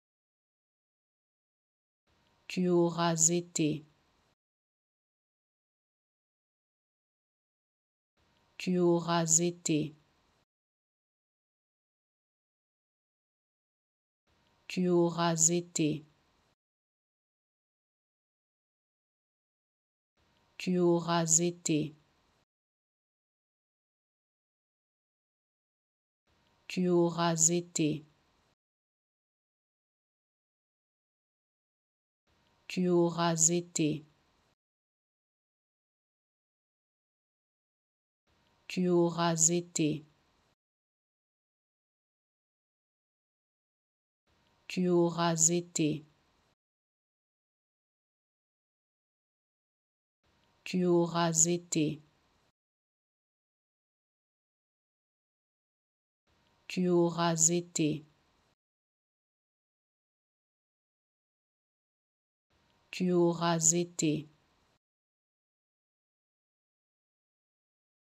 How to Pronounce ''Tu auras été'' (You will have been) Correctly in French
Learn how to say and properly pronounce ''Tu auras été'' in French with this free pronunciation tutorial. Apprenez à prononcer des mots en français grâce à nos tutoriels.